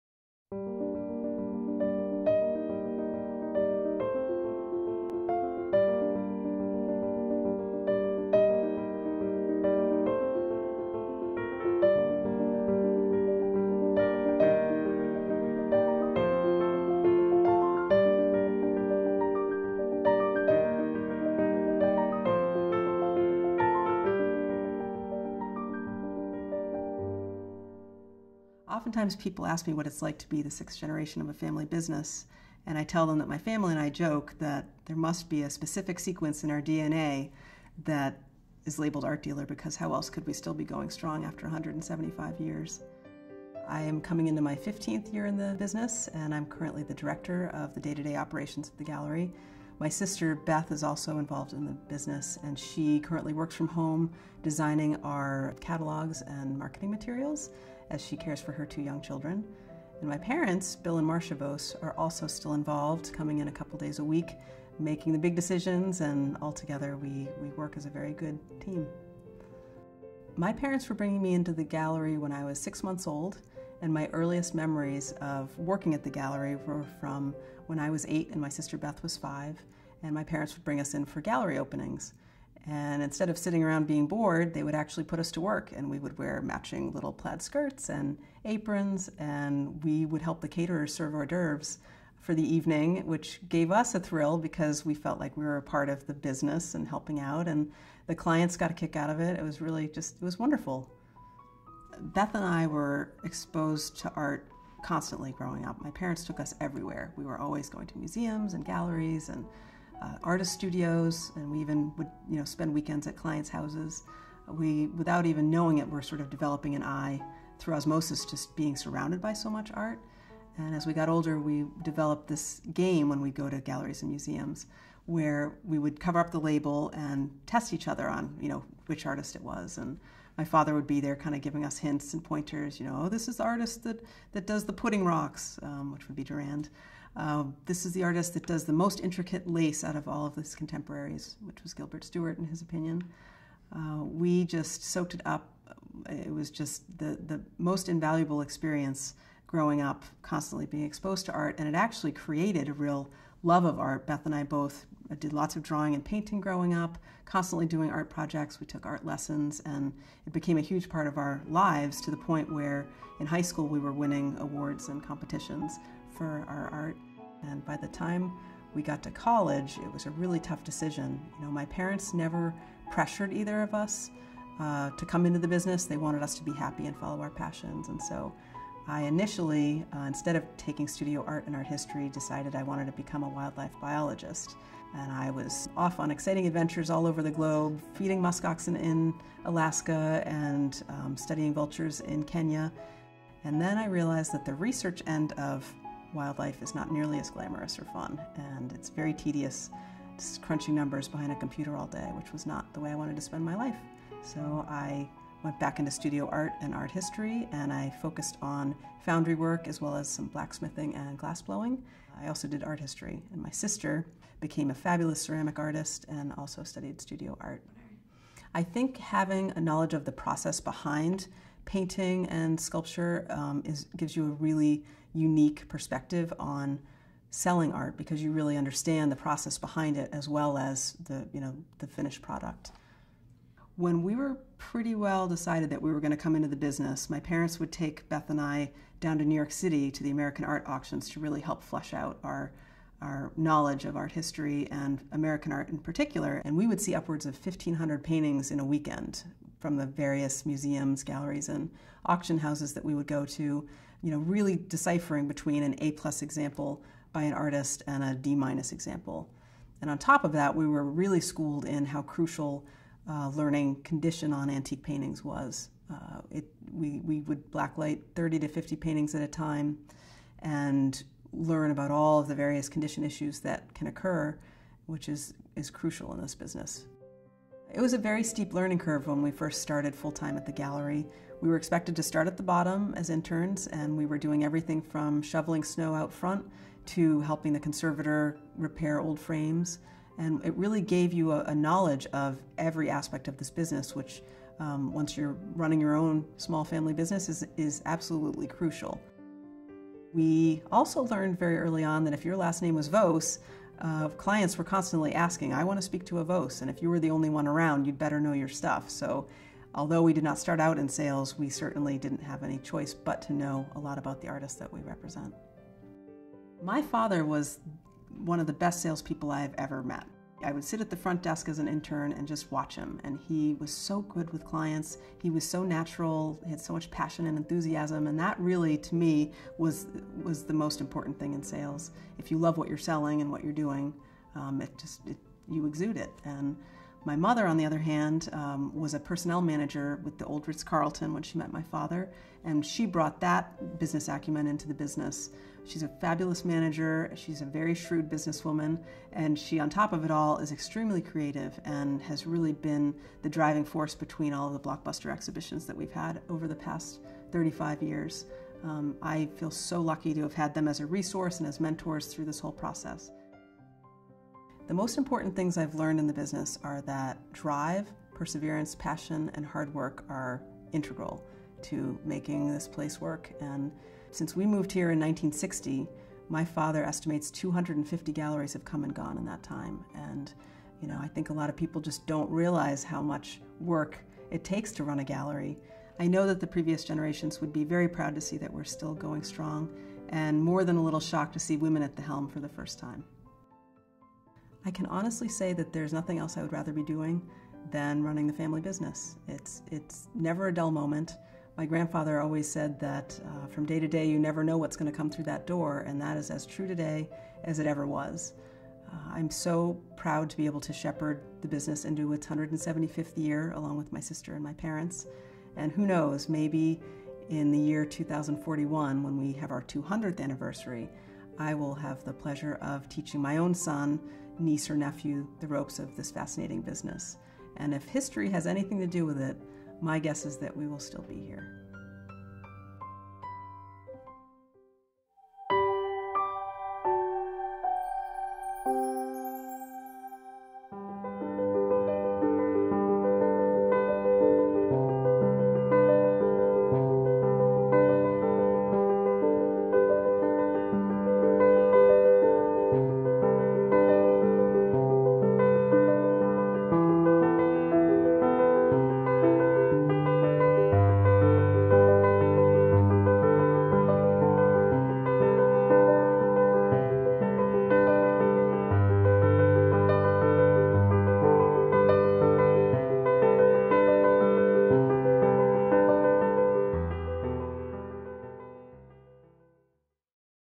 How the Sixth Generation Joined the Business
This installment of Tales of an Art Dealer features how the sixth generation of the Vose family became involved in the business. Narrated by Carey L. Vose, this video features insights on successfully passing down trade knowledge from generation to generation.